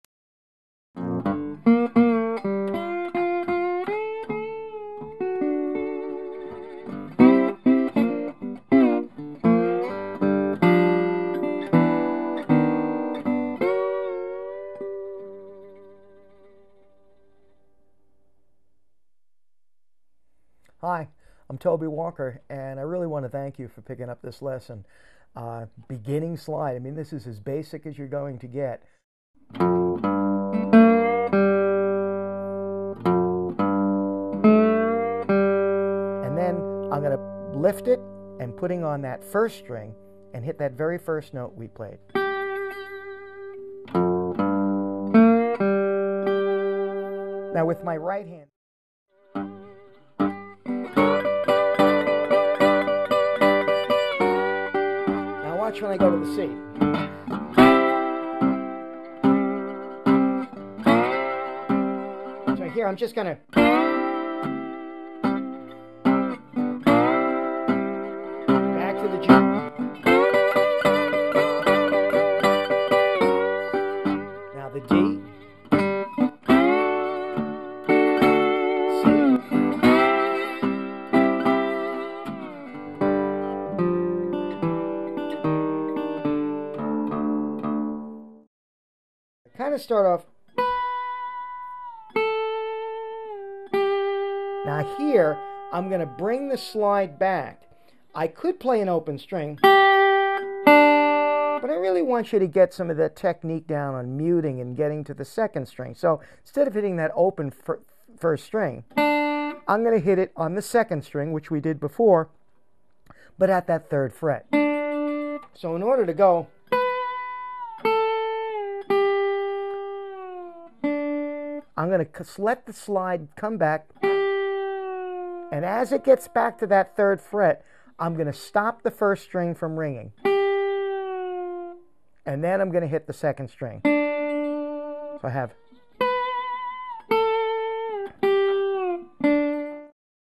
Get Started on Bottleneck Slide Guitar!
If you've always wanted to play bottleneck slide guitar, this lesson shows you how it can be done in the easiest and fastest possible way. Toby Walker's introduction is perfect for fingerstyle players who want to put real power in their picking.